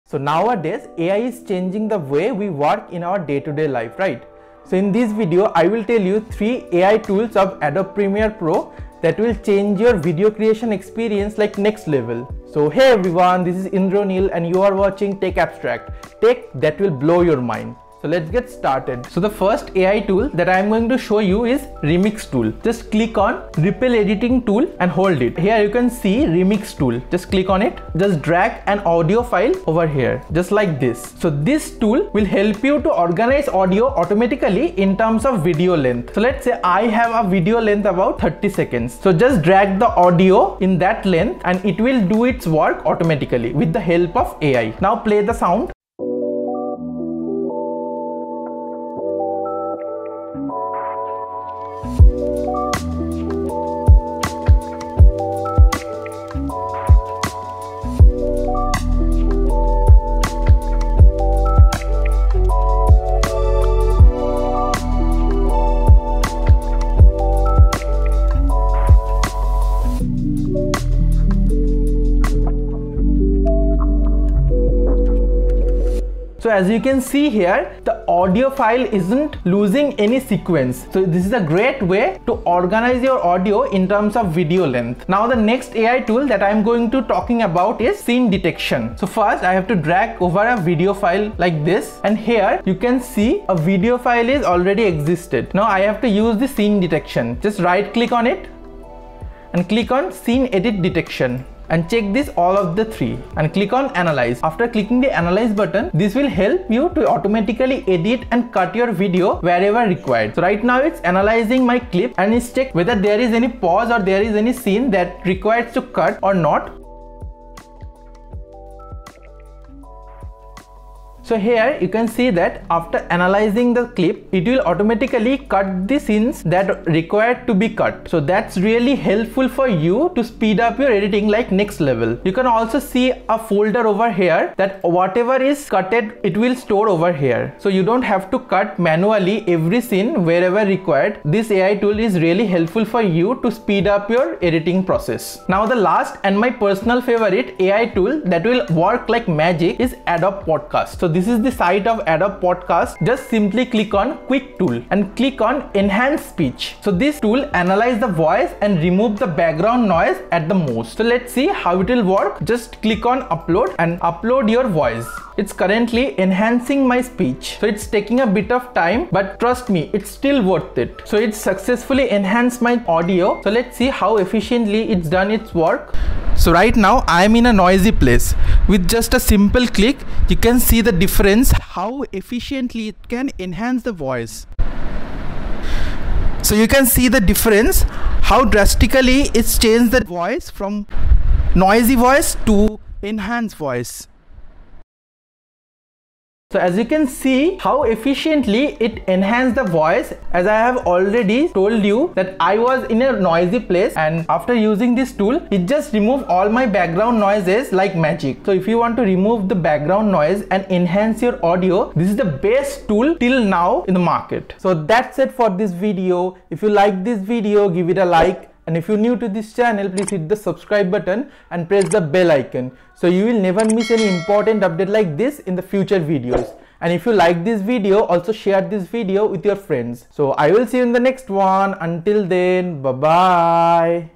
Boost Your Video Editing with These Top 3 AI Tools in Adobe Premier Pro
In this video, we'll be discussing the top 3 AI tools available in Adobe Premier Pro that can help you streamline your video editing process and produce professional-quality videos. These AI tools are designed to help you enhance your footage and speed up your workflow, so you can spend more time focusing on the creative aspects of your project. We'll walk you through each tool and demonstrate how to use them effectively, giving you the skills and knowledge you need to take your video editing game to the next level. Whether you're a seasoned editor or just starting out, these AI tools are a must-try for anyone looking to boost their editing capabilities in Adobe Premier Pro.